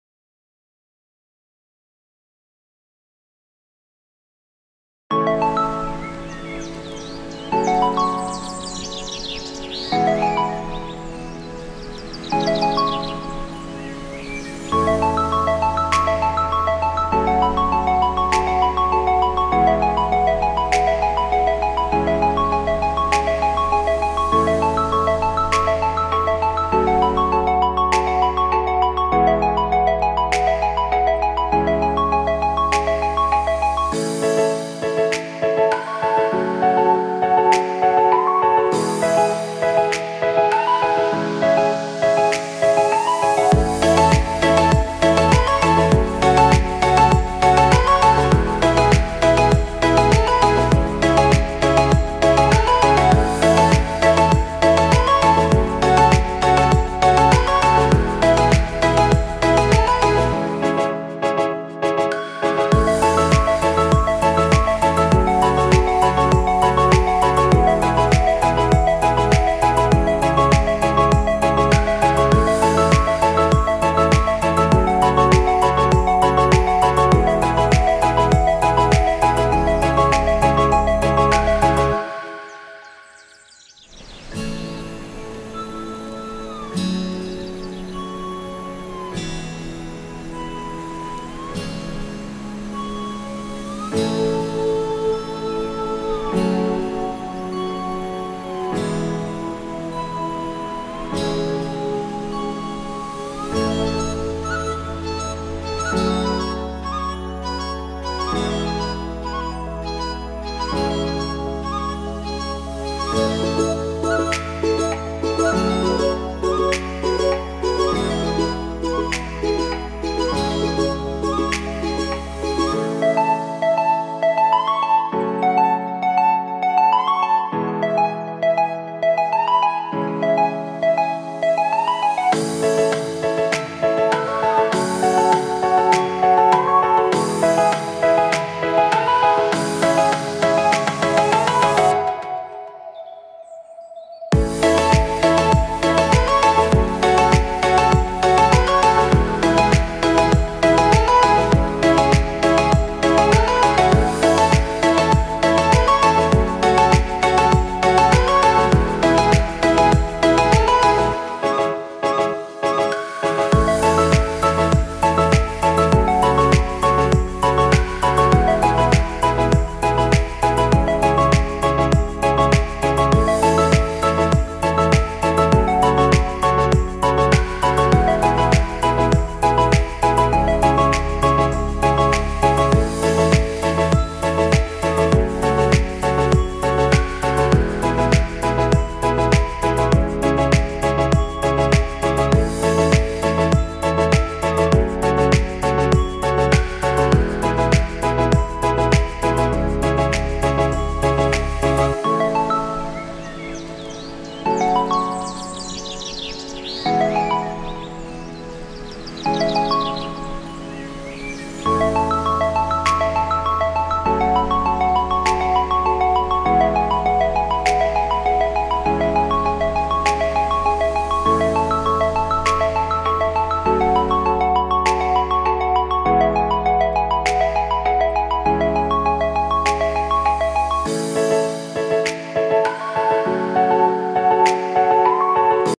how is the rose?it is just a try. craftycrew craftworks beginners youtubevideo---.
a rose made with paper for a try ,this is one of the method of making rose using paper.comment if u like it.